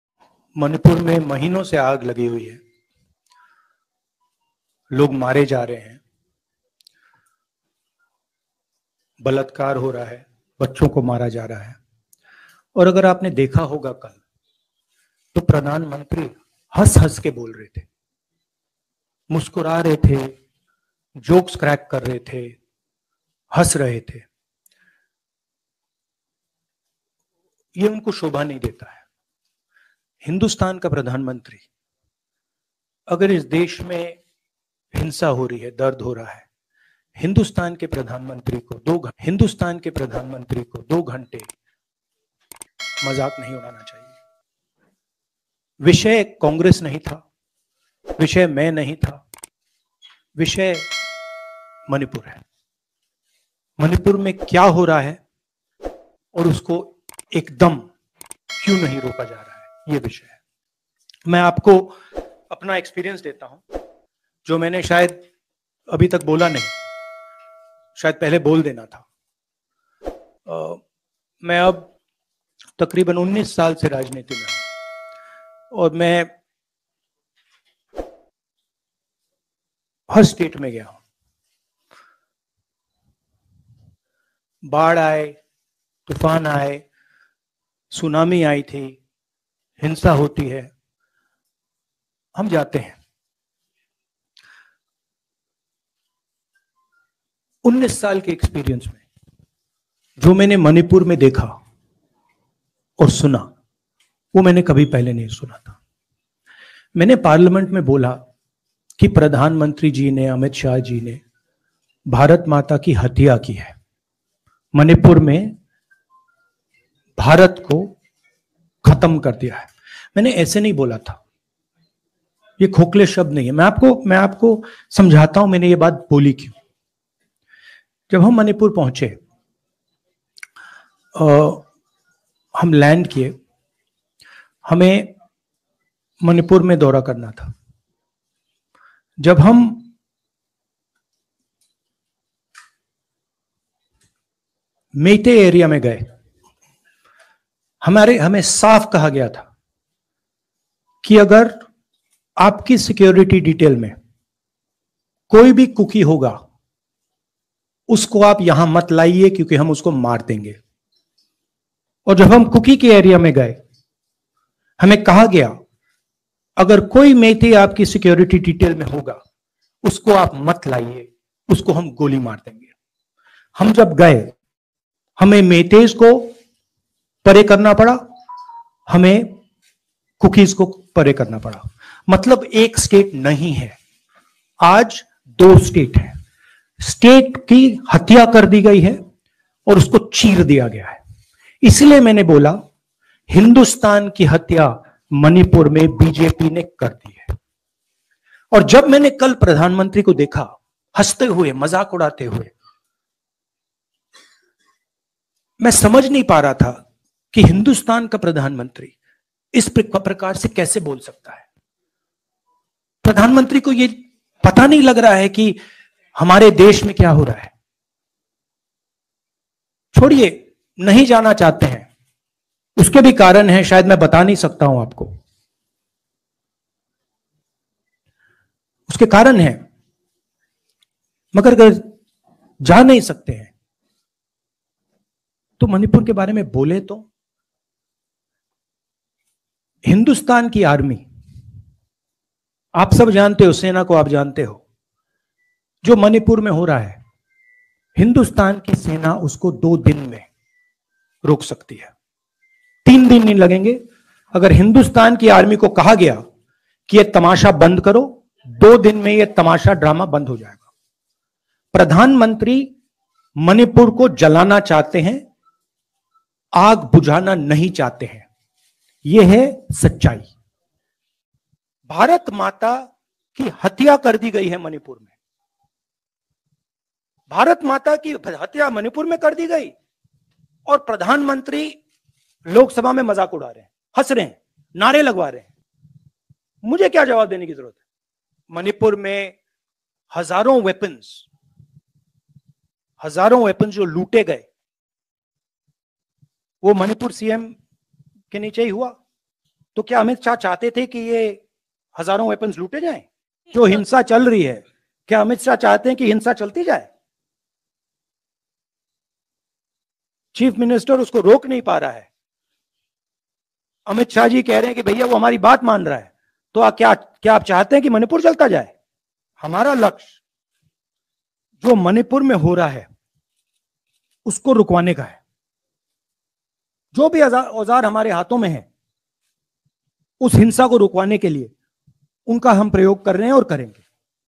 Rahul Gandhi Speech:- मणिपुर जल रहा है और पीएम मोदी हंस रहे हैं.
Rahul Gandhi Speech :-  Manipur is burning and PM Modi is laughing.